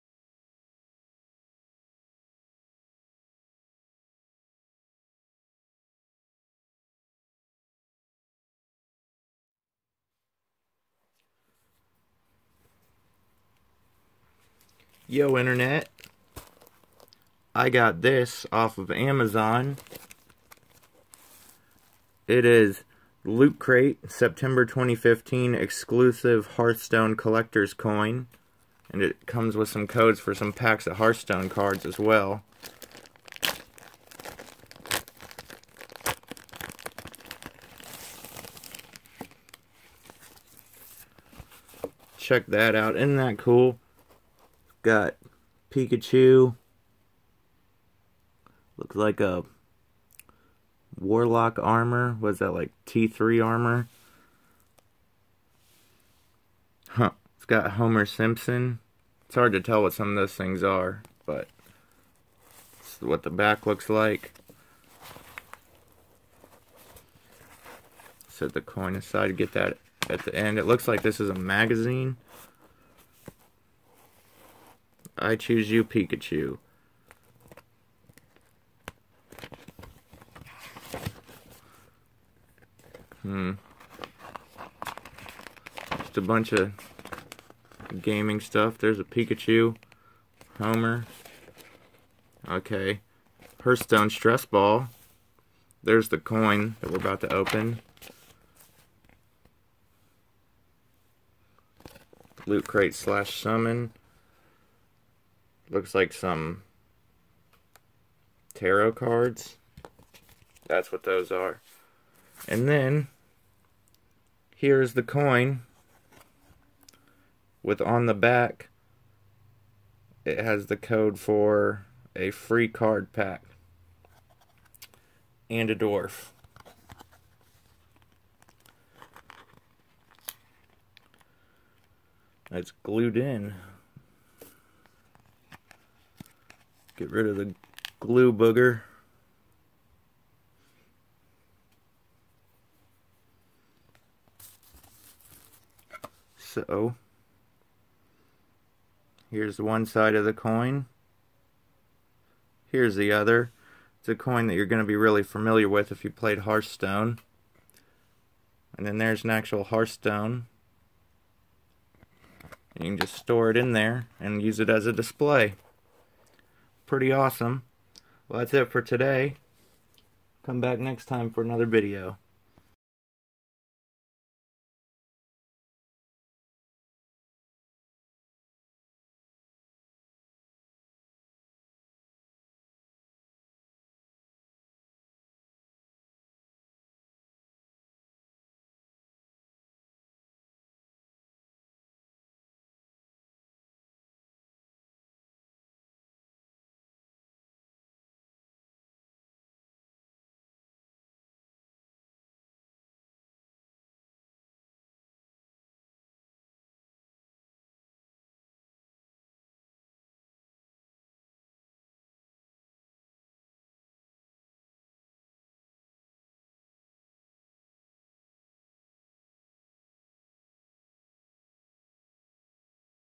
Hearthstone Coin Replica, from Loot Crate September 2015!
Opening up a Hearthstone Replica Coin! It comes with a pack of Cards in Hearthstone! You can get one for yourself at the first link below, if you would like too! Thank's for watching! Have an awesome day!